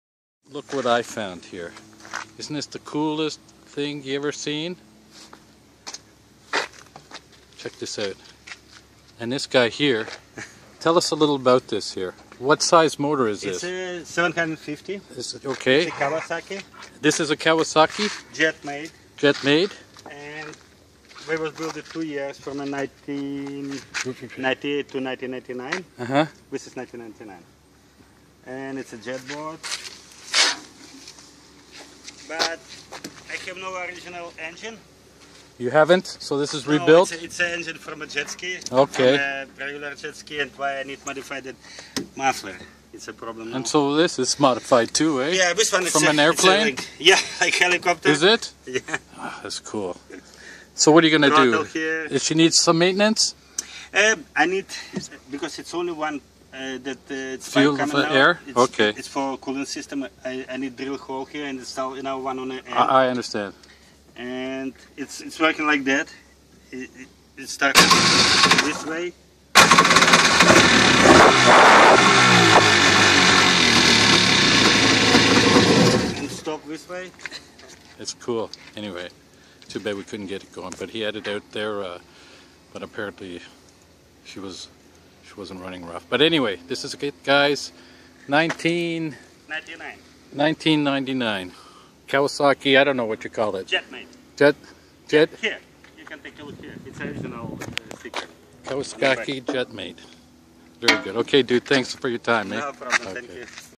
Kawasaki Jet Mate 1998
Interesting jet ski, 1st one i ever seen , kinda cool .